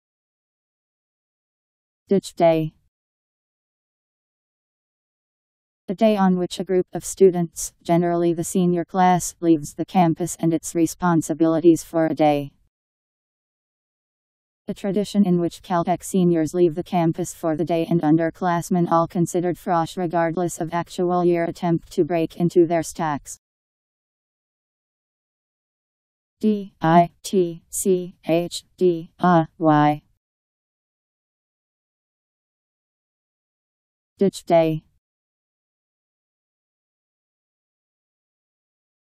Ditch day Meaning
Video shows what ditch day means. A day on which a group of students, generally the senior class, leaves the campus and its responsibilities for a day.. A tradition in which Caltech seniors leave the campus for the day and underclassmen (all considered frosh regardless of actual year) attempt to break into their stacks..  ditch day pronunciation. How to pronounce, definition by Wiktionary dictionary. ditch day meaning. Powered by MaryTTS